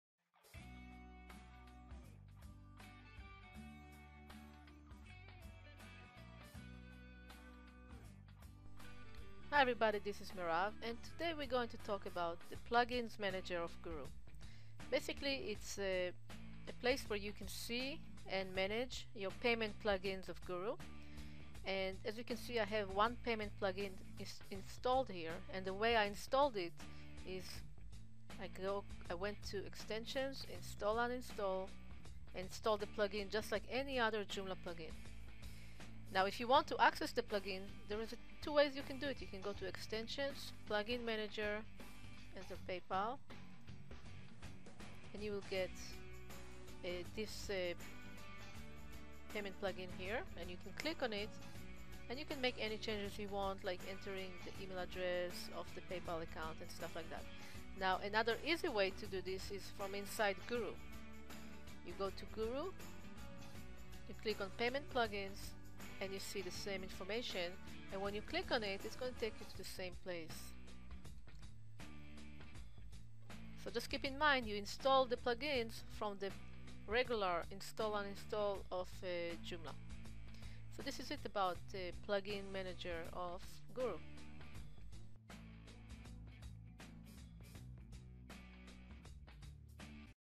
Joomla LMS - Guru - Payment Settings
This video explains about the payment settings of Guru - Joomla LMS.